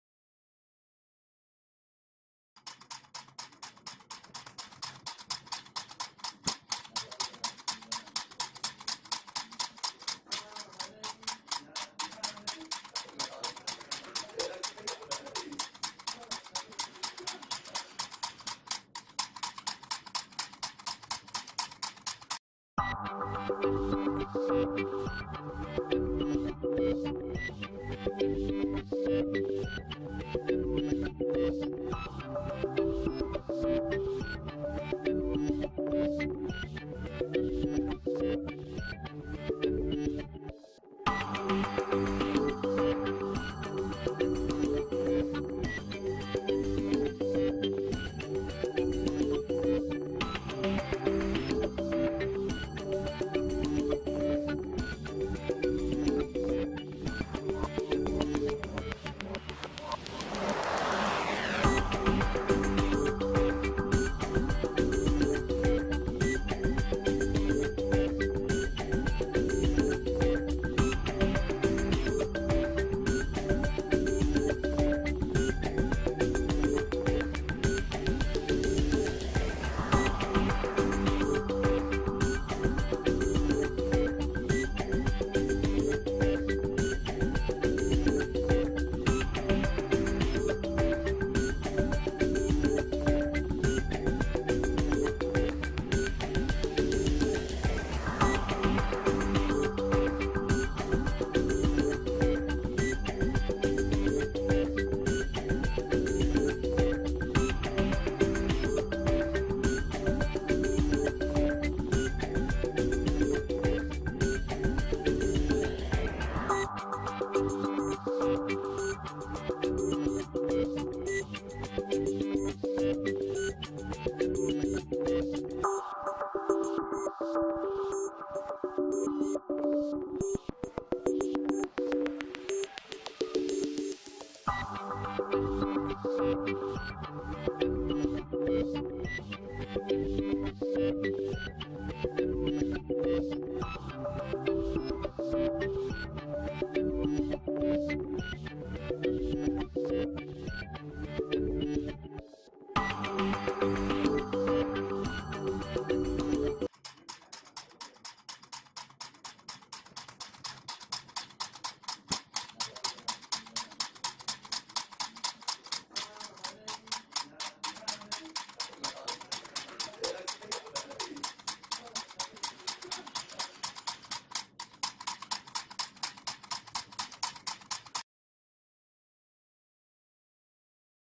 How to make a Solenoid valve engine/motor DIY
Hello friends, in this video i have try to make a solenoid engine

below are the link of more interesting video

You can also find more interesting videos related to quadcopter or drone making in below link

robotronics